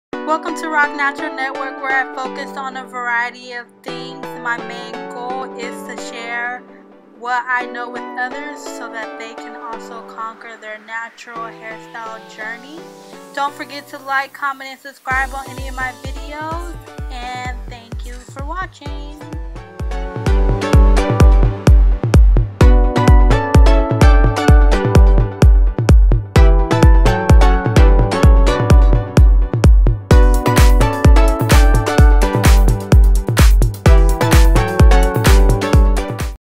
See What My Page Is About| Trailer
Hey Yall,
Here is a video that demonstrates what my page will be focusing on. I needed a marketing piece for a competition so I took this as a great opportunity to create a "trailer" for my page! Also as my page continues to grow, my new subscribers will get a glimpse from the top what my page is about. As well as people are able to tell what I'm about.